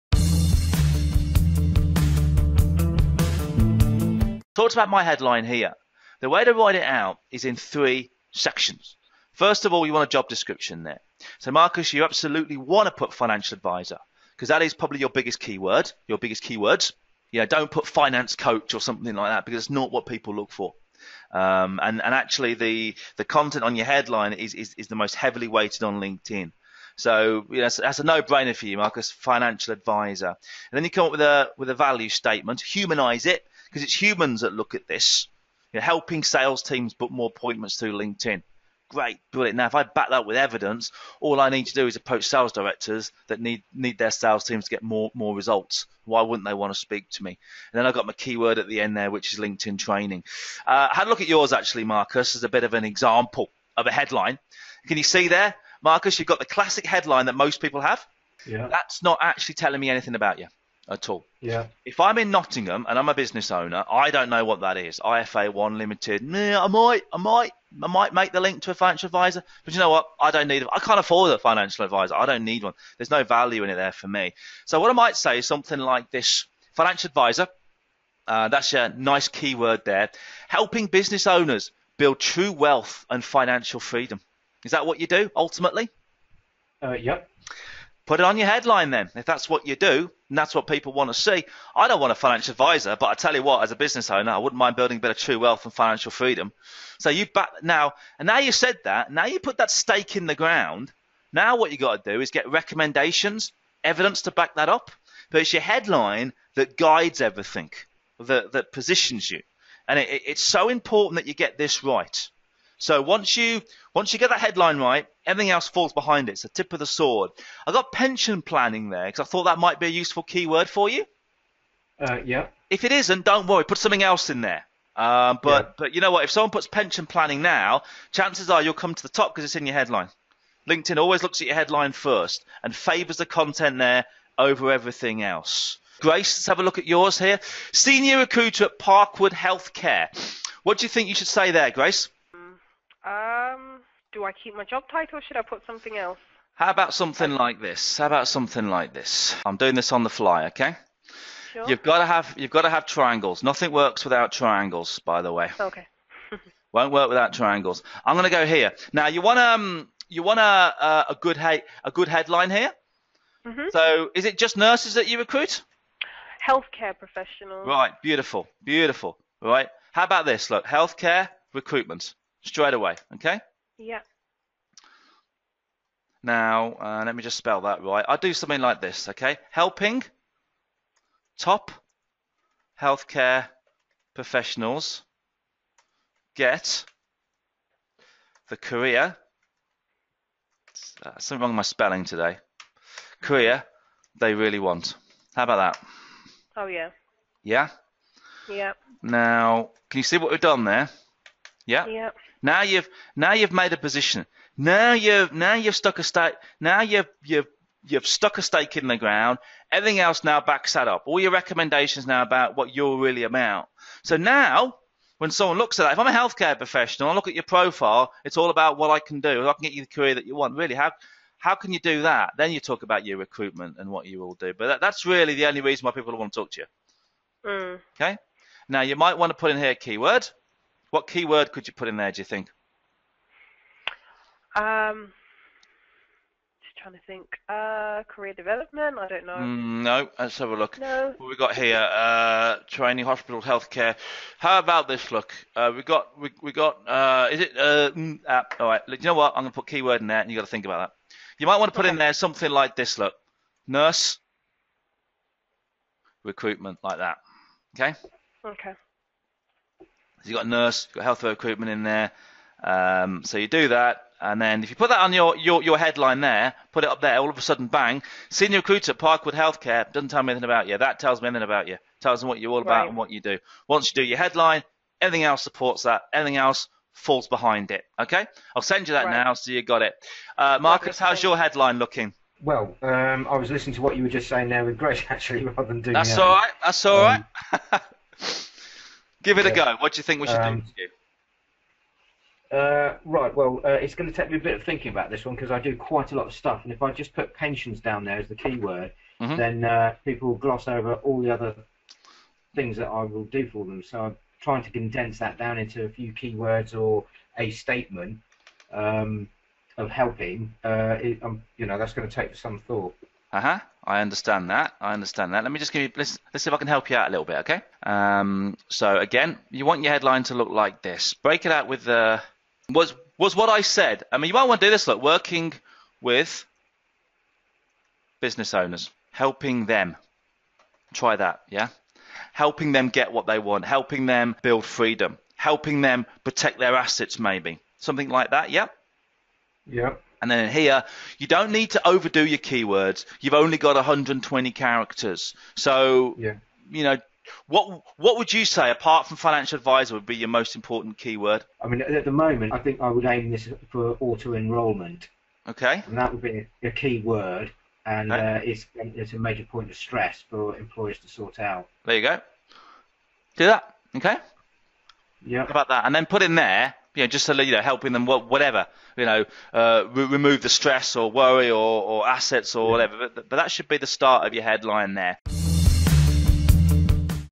Great LinkedIn Profiles - Creating Your Professional Headline
Creating Your LinkedIn Professional Headline

*The key to creating your Linkedin professional headline is to workout how it is you want to engage, then work out your value to them*